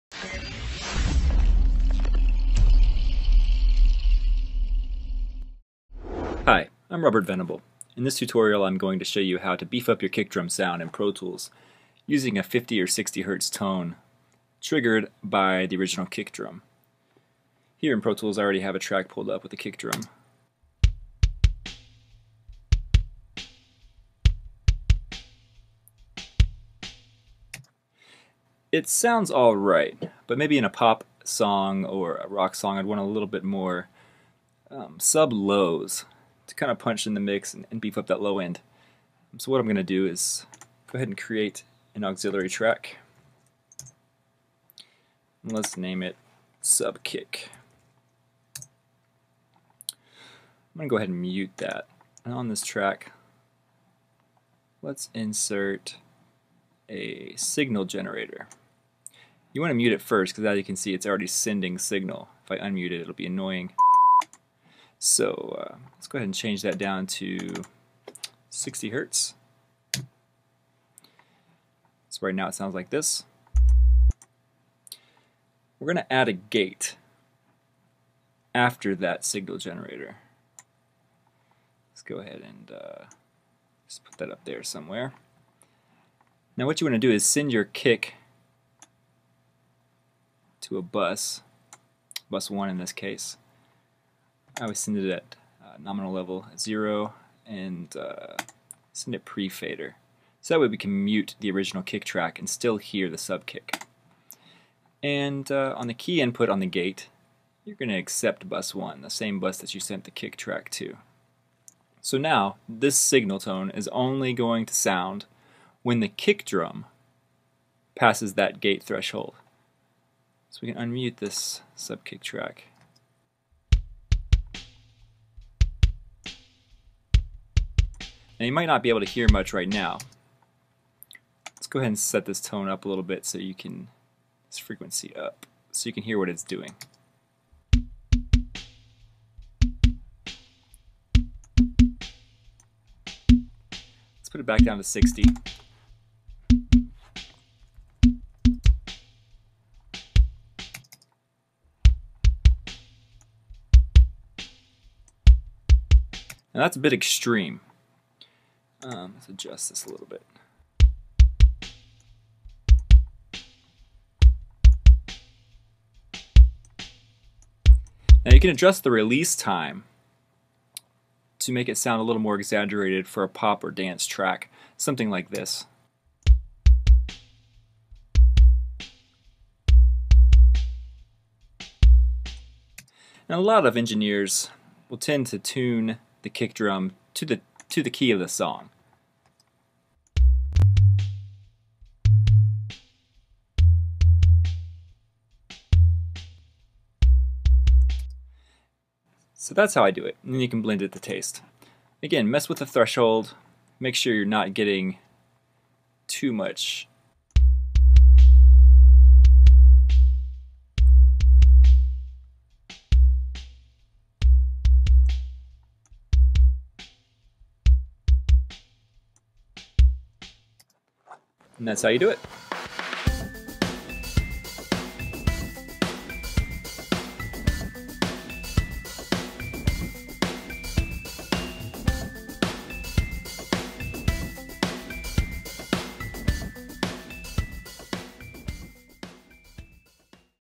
How to make your kick drum sound HUGE in the mix
This tutorial will show you how to use your existing kick drum track to trigger a signal generator and make your kick sound HUGE!  I'm using Pro Tools but you can use any DAW to do this simple trick.